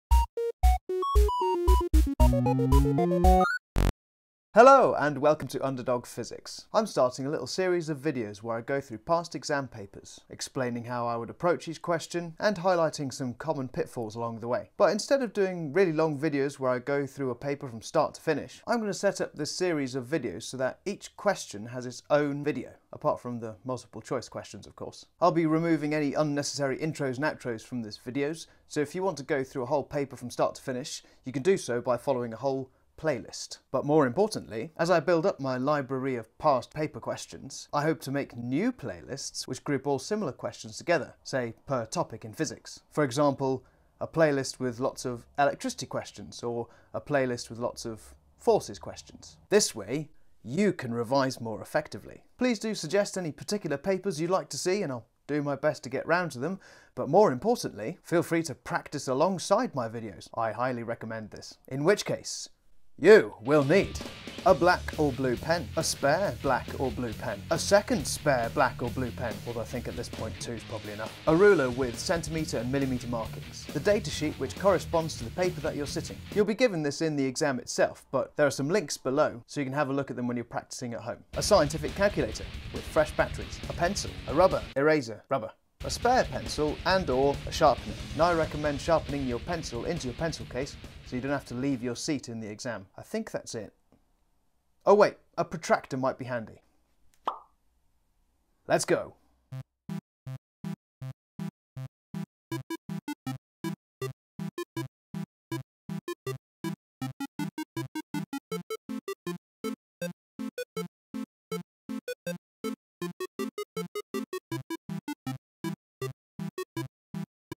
Revising for exams using past papers - GCSE (and A Level) Physics
I'm adding new series of videos to my channel covering past papers. I hope that grouping them in the way I plan to will help you find the questions/topics you need to focus your revision.

Data sheets for various exam boards and levels:
GCSE:

00:04 - Hello!
00:14 - Rationale
01:02 - YOU will need!
01:40 - Exam playlists/videos (stay tuned for more!)